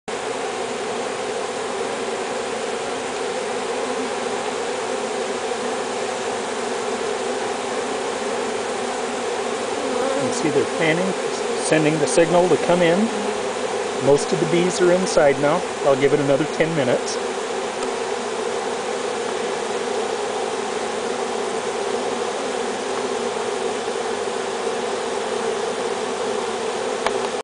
Honey Bee Swarm Capture III
April 10, 2007 - The third of five short videos showing a swarm move into a new hive.  Most of the bees have already entered and there are many bees at the entrance fanning their wings to send a signal to the others to join with them inside.  Nature at its best.